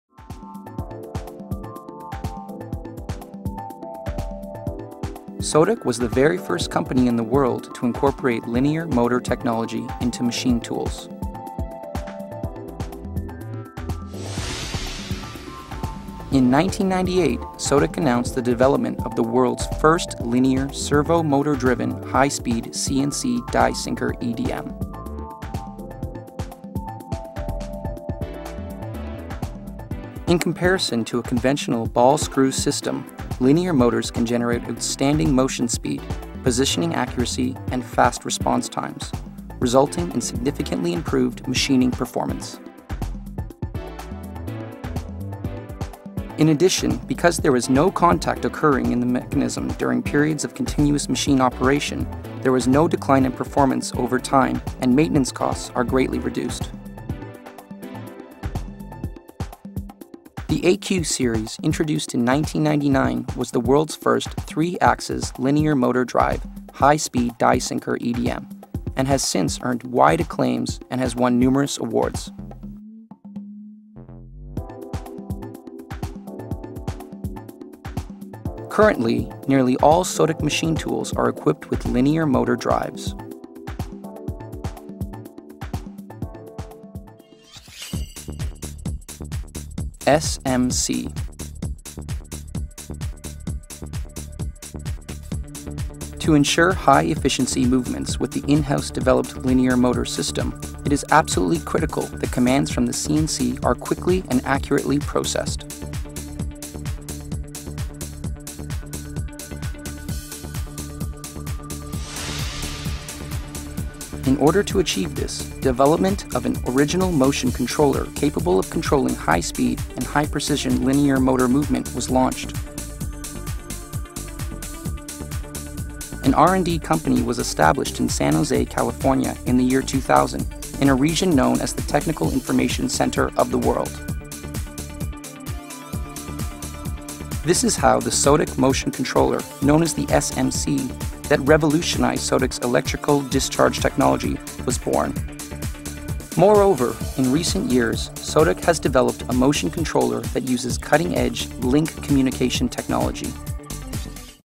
Sodick's Linear Motor Drive and Motion Controller Development
This video explains the relationship between Sodick's linear motor drives and its motion controller.

When Sodick switched to linear motor drives in 1998, they also switched from motor drivers to a motion controller which maximized the speed and acceleration of the linear motor drives.